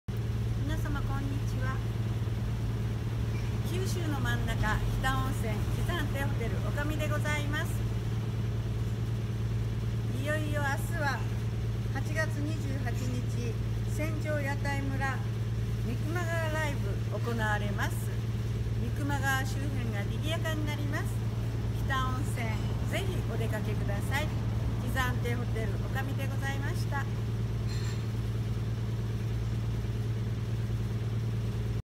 【ごめんなさい8/27 14:35 中止になりました】いよいよ明日は日田温泉まつり2016 船上屋台村＆三隈川ライブ
【ごめんなさい8/27 14:35 中止になりました】

いよいよ明日は日田温泉まつり2016 船上屋台村＆三隈川ライブ

隈町公園と屋形船で開催される
日田温泉まつり2016 船上屋台村＆三隈川ライブのご案内です。
日時：８月２８日（日）１３時～２０時
入場無料 ＠隈町児童公園＋屋形船
屋形船出店
・あじな　・いちふく　・天領酒場三丁目
・チャビー　・ぶっかけ亭　・三隈飯店
・ステップす　・寿家支店中央店 and more

ステージイベント
そらは金色
侍 BALLERS
バイコージャックソン
スカロケッツ
THAMII
ピストルパッキンママ
etc..

同時開催 フリーマーケット
エホントマルシェとろ箱市
主催 三隈川昭和会 協賛 隈町商店街